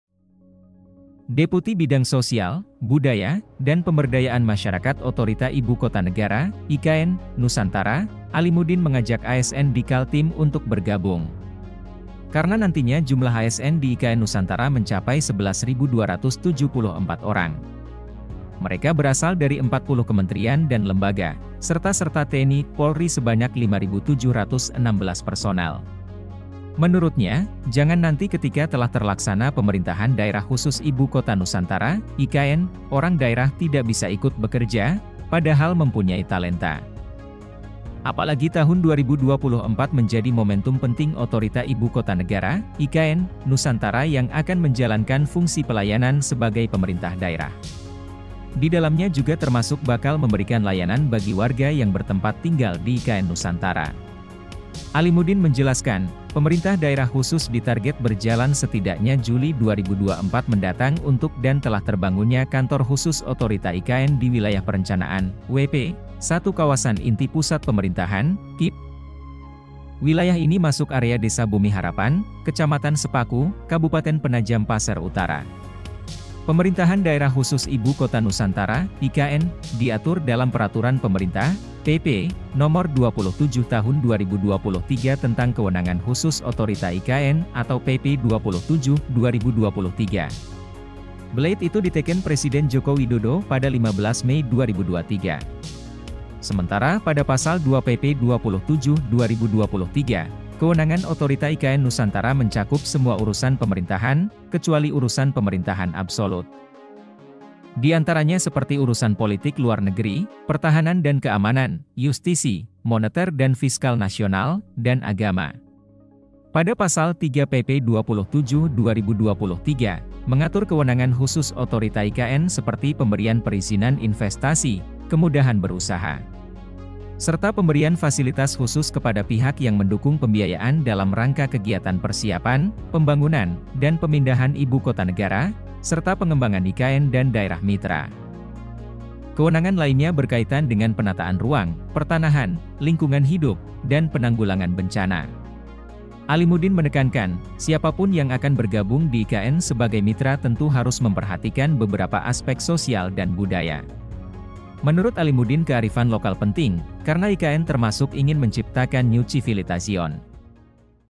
Deputi Ajak ASN di Kaltim untuk Bergabung di Otorita IKN Nusantara
Deputi Bidang Sosial, Budaya, dan Pemberdayaan Masyarakat Otorita Ibu Kota Negara (IKN) Nusantara, Alimuddin mengajak ASN di Kaltim untuk bergabung.
Karena nantinya jumlah ASN di IKN Nusantara mencapai 11.274 orang.
Mereka berasal dari 40 kementerian dan lembaga, serta serta TNI/Polri sebanyak 5.716 personel.
Menurutnya, jangan nanti ketika telah terlaksana Pemerintahan daerah khusus Ibu Kota Nusantara (IKN) orang daerah tidak bisa ikut bekerja, padahal mempunyai talenta.
Apalagi tahun 2024 menjadi momentum penting Otorita Ibu Kota Negara (IKN) Nusantara yang akan menjalankan fungsi pelayanan sebagai pemerintah daerah.

Editor: vp05_Jf